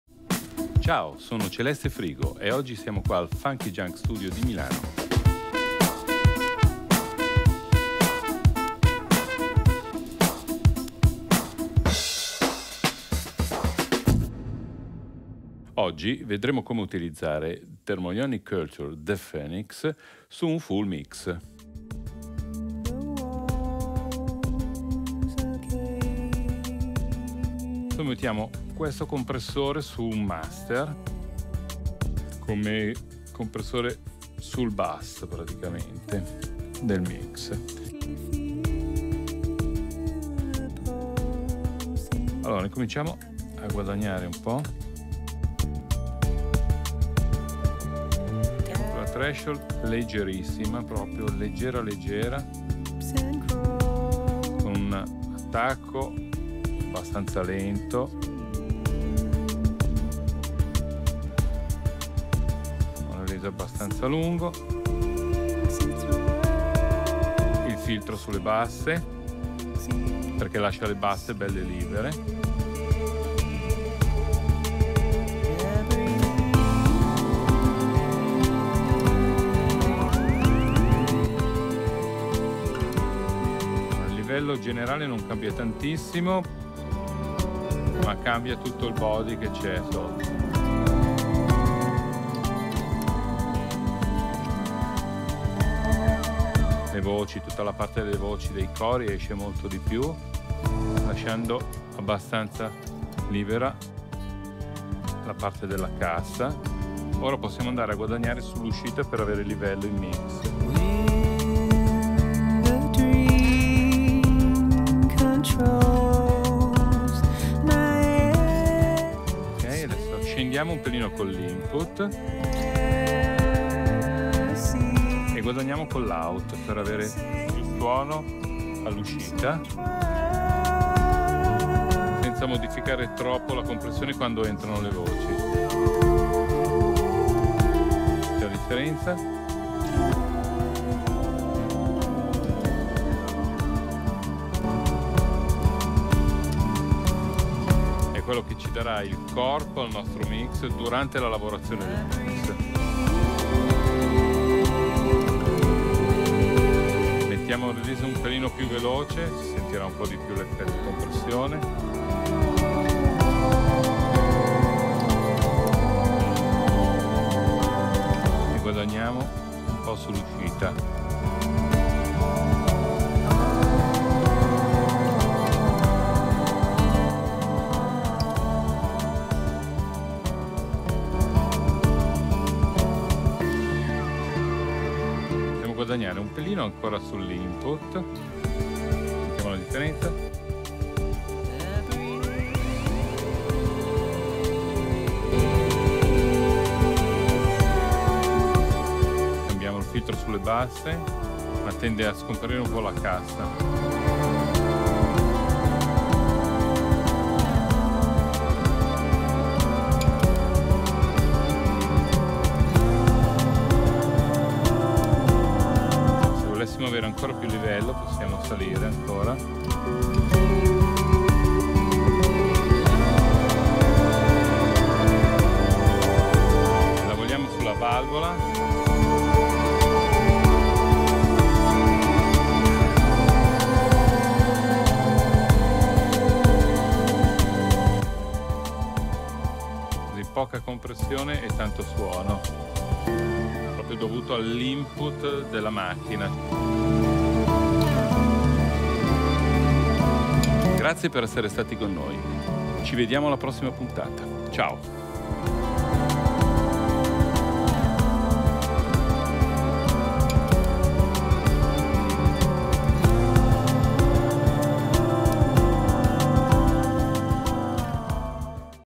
PRO TIPS - The Phoenix Mastering Plus: Full Mix - con Celeste Frigo
Celeste Frigo, mix engineer che vanta collaborazioni del calibro di Fabrizio De Andrè, Mina, Jannacci, Subsonica, Bluvertigo, Morgan, Toto Cutugno, Miguel Bosè e Gianluca Grignani, ci mostra come usare al meglio The Phoenix Mastering Plus.